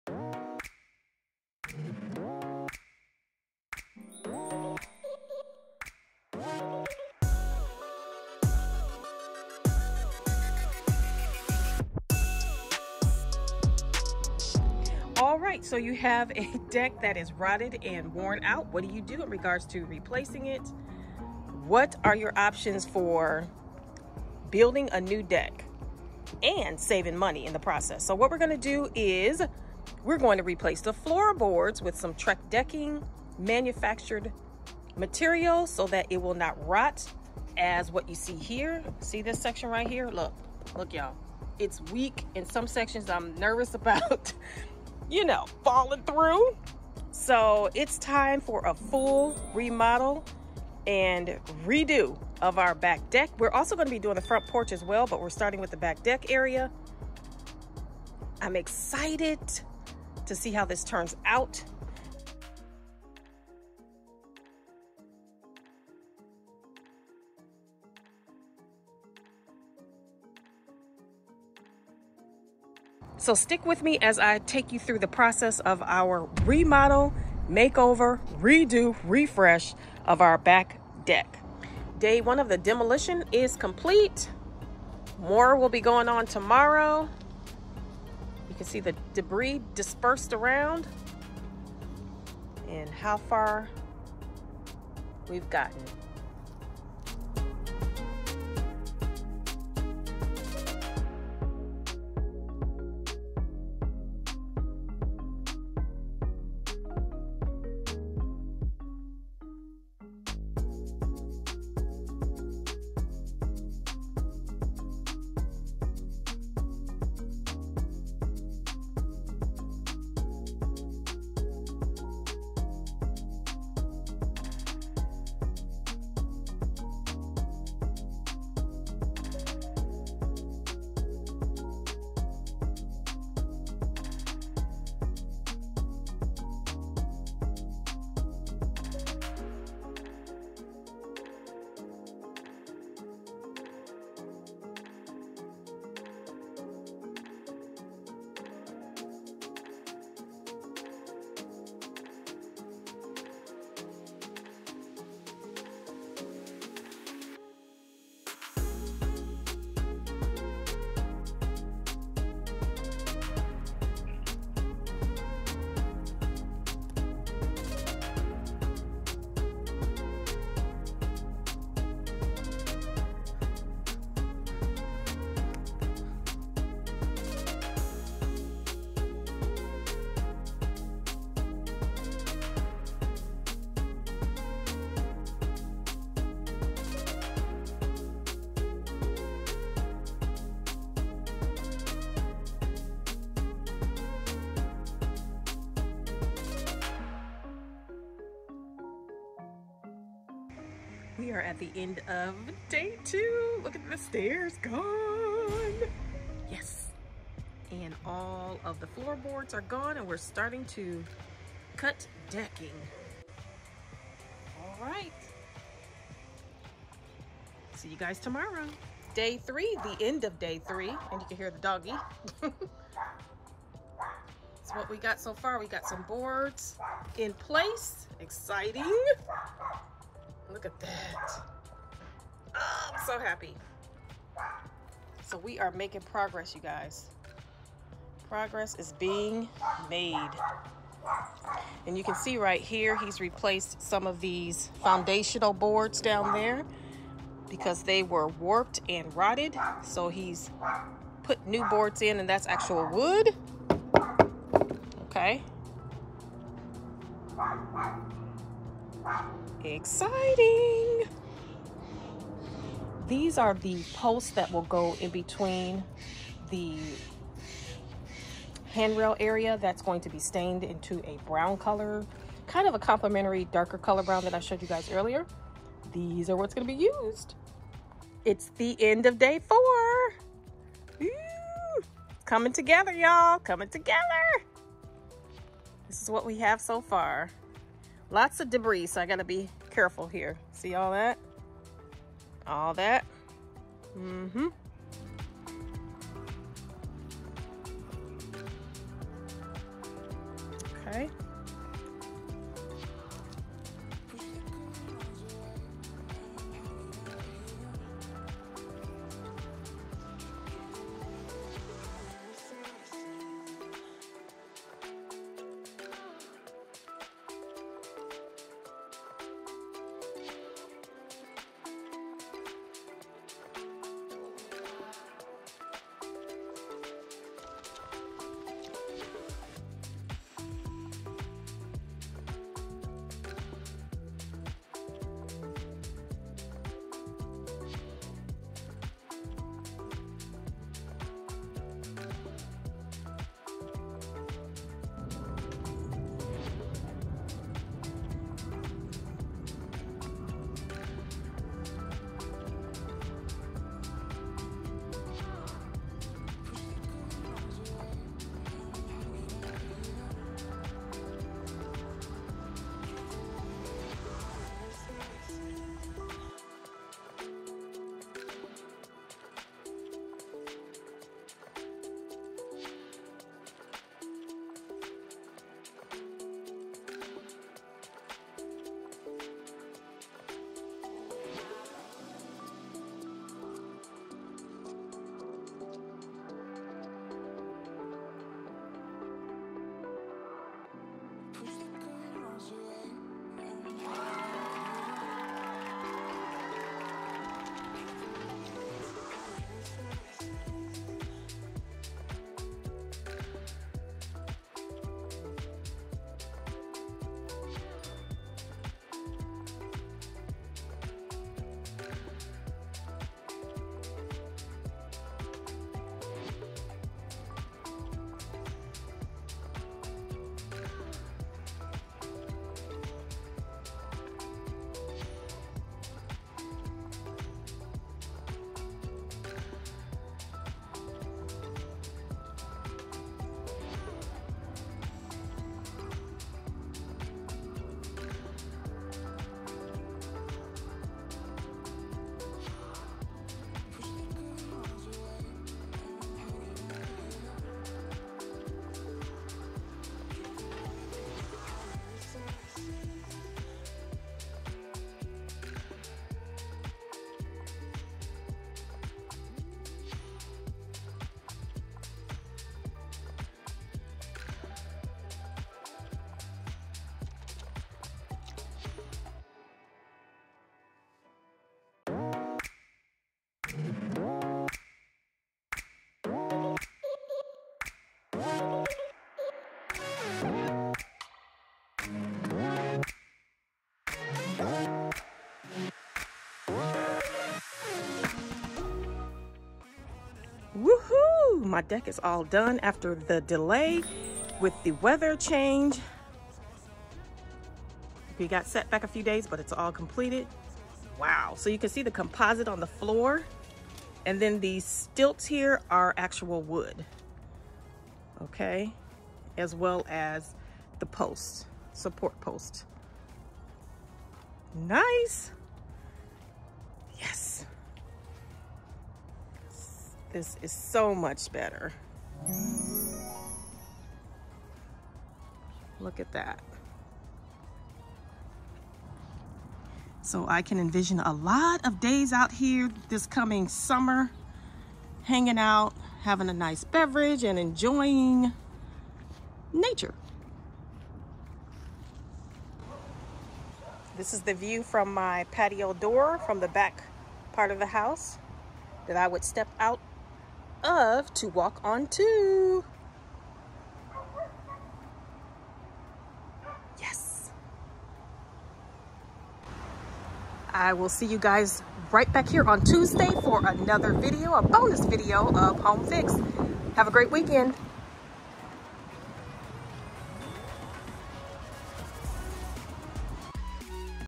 Deck Remodel Reveal | Process | 2020
Deck Remodel | Before & After | Process | 2020

Watch as I take you along the process of my brand new deck remodel with before and after footage.  At the end, I will reveal to you the final result of this multi-day process. A mixture of manufactured material & natural wood were used to complete this project. This is a deck remodel show & tell that is both satisfying and motivating to watch.  Deck remodel | reveal before & after | process | 2020